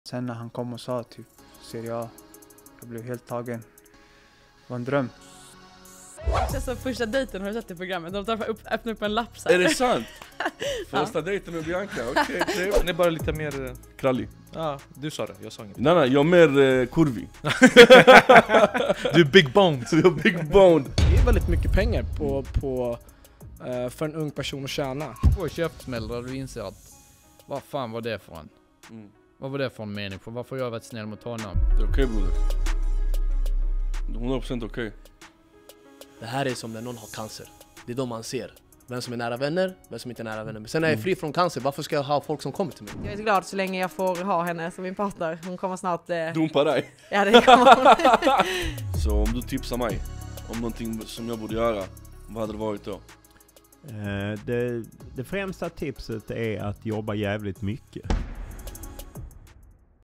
Heltidsproffs officiella trailer!
Podcasten där vi möter heltidsproffs som delar med sig av sitt livs historia och det där steget som krävs från att ta sin dröm från amatör till proffs!